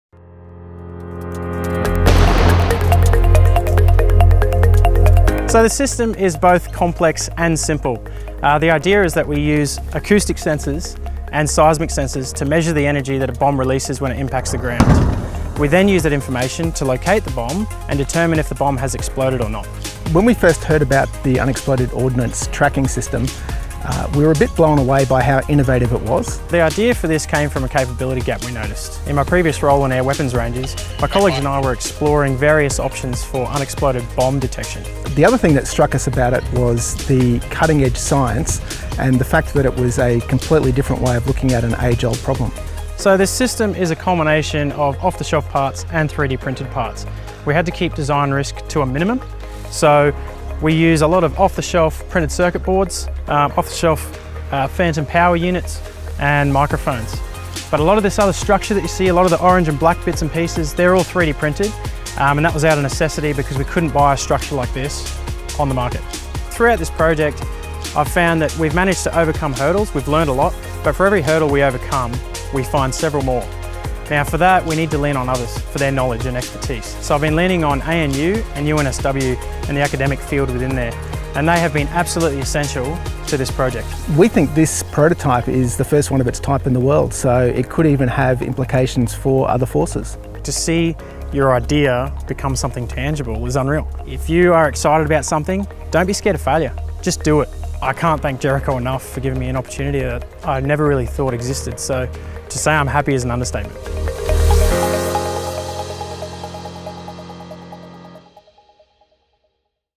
UXO Tracks - Developing a new way of finding unexploded ordnance
Blown away by innovation.

▶️ WATCH | Locating unexploded bombs on weapons ranges is risky and time consuming for Explosive Ordnance Disposal (EOD) crews. We're working with ANU and UNSW to develop a ground-breaking solution using seismic and acoustic sensors to determine where a bomb hits the ground, and if it has exploded.

The first of its kind in the world, UXO Tracks is set make waves; reducing the risk and cost for explosive ordinance disposal crews.